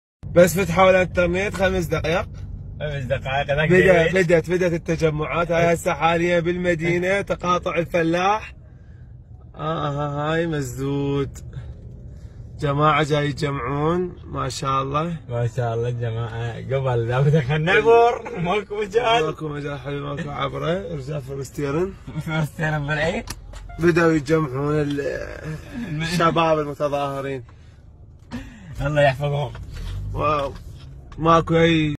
متداول | المتظاهرون يغلقون الطرقات الرئيسة في مدينة الصدر شرقي بغداد بالإطارات المشتعلة
متداول | المتظاهرون يغلقون الطرقات الرئيسة في مدينة الصدر شرقي بغداد بالإطارات المشتعلة في سابع أيام
ــــــــــــــــــــــــــــ